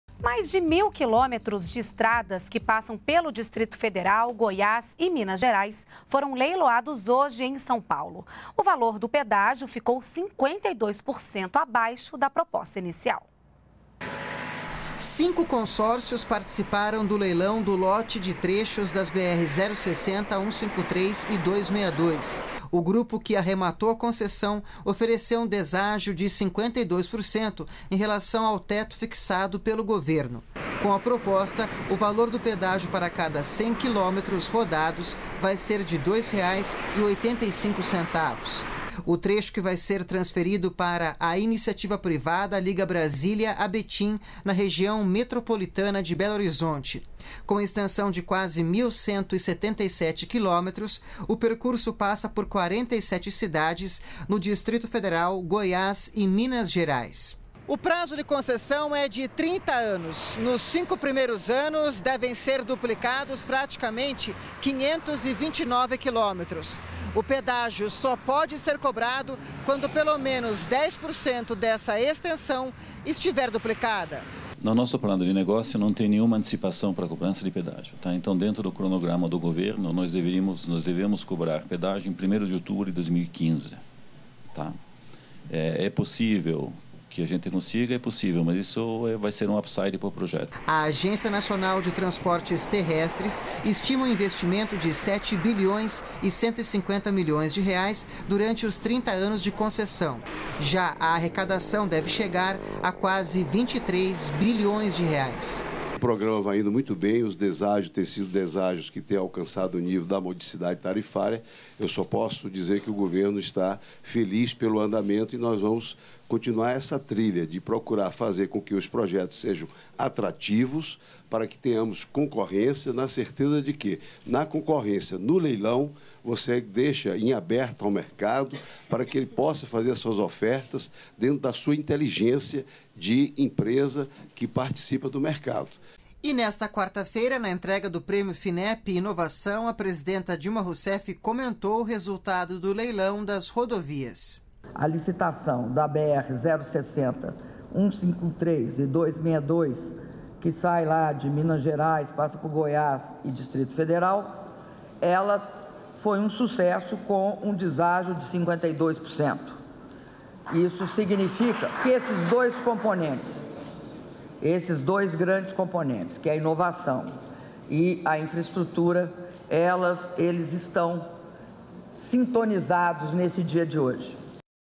Trechos de 3 BRs são concedidos à iniciativa privada com deságio de 52% no pedágio
NBR NOTÍCIAS - 04.12.13: Nesta quarta-feira (4), foi anunciado o vencedor do leilão para a concessão das rodovias BR-060, BR-153 e BR-262 no Distrito Federal, Goiás e Minas Gerais. A empresa vencedora apresentou a proposta de pedágio de R$ 2,85 para cada 100 quilômetros rodados, valor 52% menor que o previsto pelo governo. A concessão vai durar 30 anos, mas a empresa só pode começar a cobrar o pedágio depois que 10% das obras estejam concluídas.